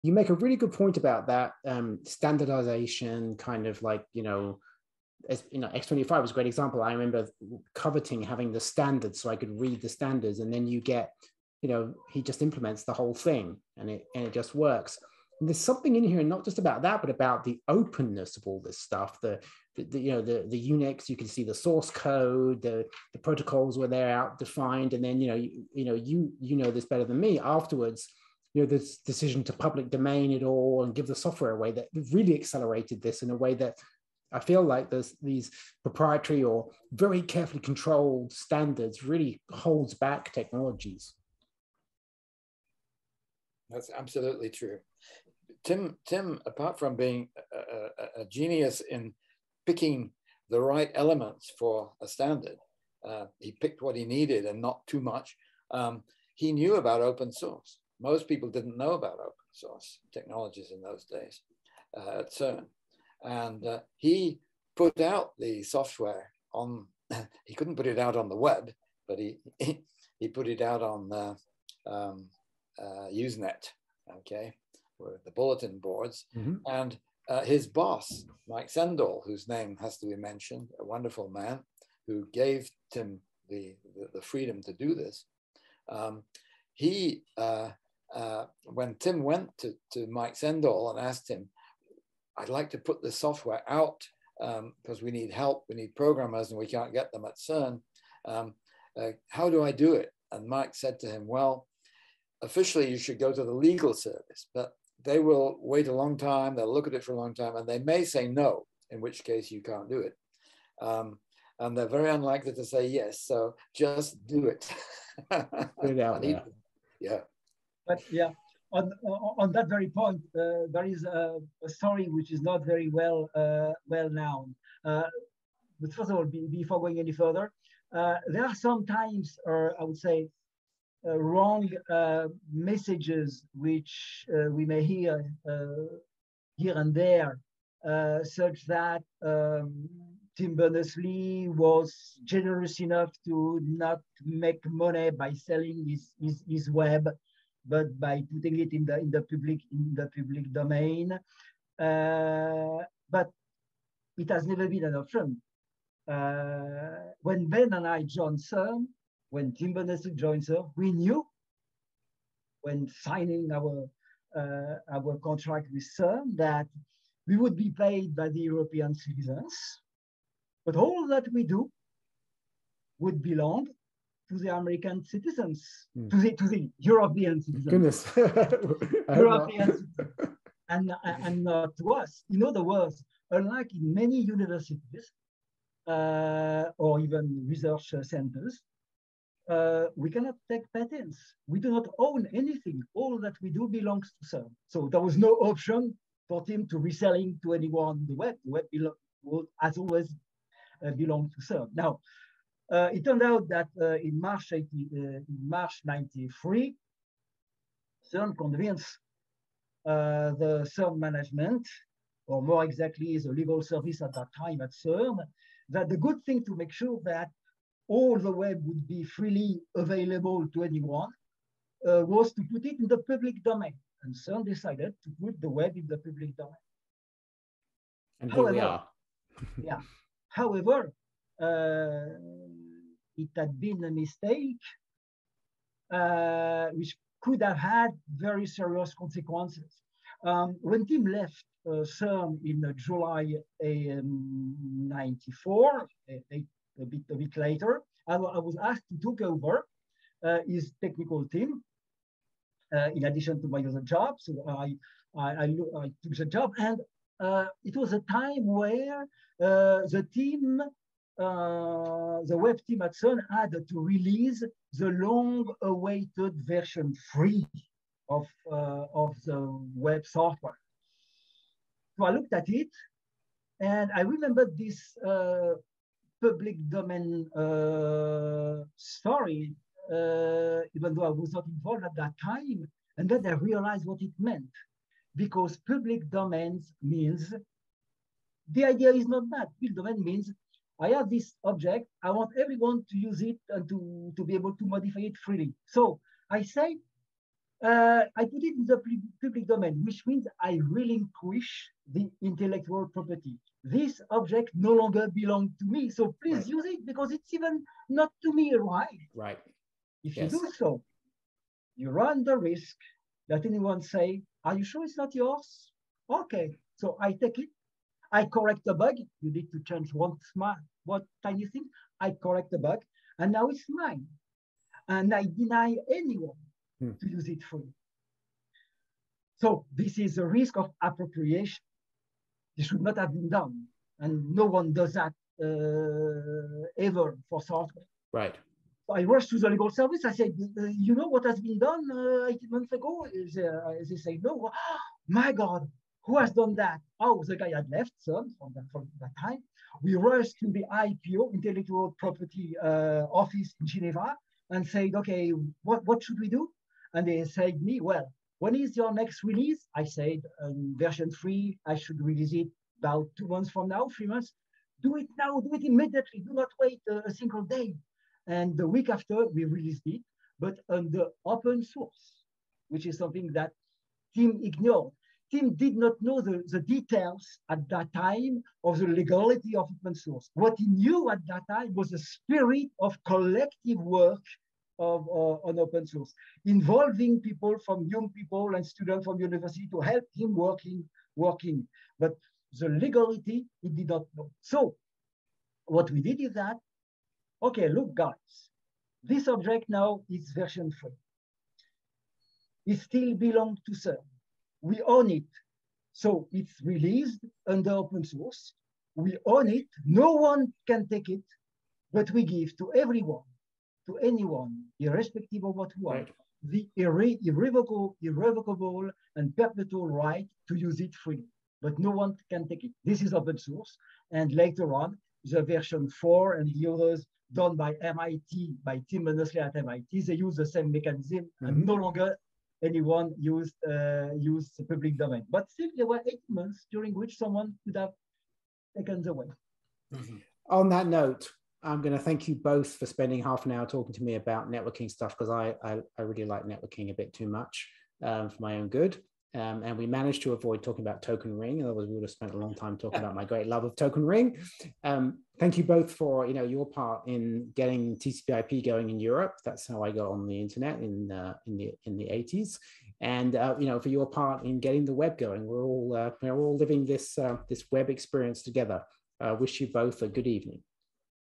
How the World Wide Web became an open source project
Do you know how the World Wide Web became an open source project? Watch Dr. Ben Segal, CERN 's first official TCP/IP Coordinator, and Francois Fluckiger, director of the CERN School of Computing, talking to Cloudflare's CTO, John Graham-Cumming, on CloudflareTV about it